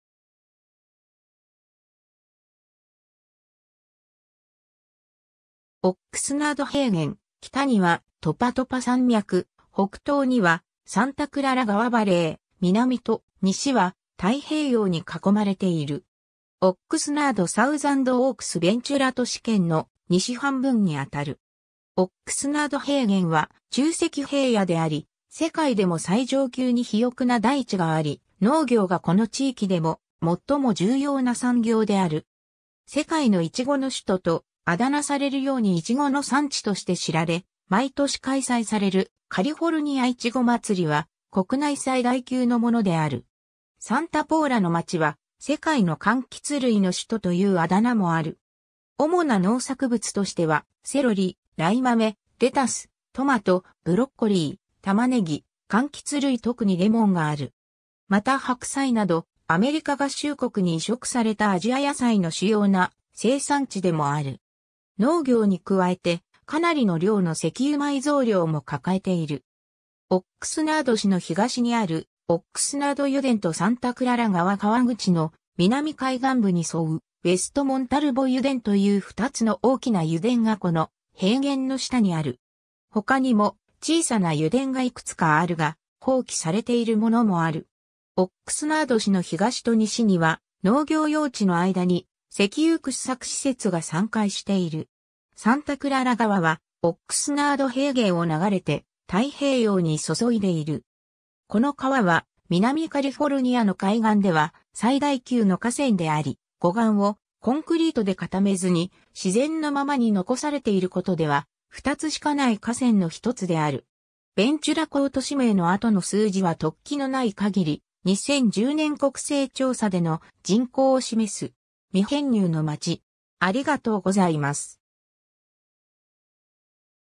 オックスナード平原
オックスナード平原（英: Oxnard Plainは、アメリカ合衆国カリフォルニア州南部のベンチュラ郡南西部にある大きな海岸平原である。東にはサンタモニカ山脈、サンタスザーナ山脈およびオークリッジ（その向こうにはコネホ・バレーがある）、北にはトパトパ山脈、北東にはサンタクララ川バレー、南と西は太平洋に囲まれている。
オックスナード・サウザンドオークス・ベンチュラ都市圏の西半分にあたる。
 オックスナード平原は沖積平野であり、世界でも最上級に肥沃な大地があり、農業がこの地域でも最も重要な産業である。
「世界のイチゴの首都」とあだ名されるようにイチゴの産地として知られ、毎年開催されるカリフォルニア・イチゴ祭は国内最大級のものである。
サンタポーラの町は「世界の柑橘類の首都」というあだ名もある。
主な農作物としては、セロリ、ライマメ、レタス、トマト、ブロッコリー、タマネギ、柑橘類特にレモンがある。
またハクサイなど、アメリカ合衆国に移植されたアジア野菜の主要な生産地でもある。
 農業に加えて、かなりの量の石油埋蔵量も抱えている。
オックスナード市の東にあるオックスナード油田とサンタクララ川河口の南海岸部に沿うウェストモンタルボ油田という2つの大きな油田がこの平原の下にある。
他にも小さな油田が幾つかあるが、放棄されているものもある。
オックスナード市の東と西には農業用地の間に石油掘削施設が散開している。
 サンタクララ川はオックスナード平原を流れて太平洋に注いでいる。
この川は南カリフォルニアの海岸では最大級の河川であり、護岸をコンクリートで固めずに自然のままに残されていることでは、2つしかない河川の1つである。
 ベンチュラ港 都市名の後の数字は特記の無い限り、2010年国勢調査での人口を示す。
 未編入の町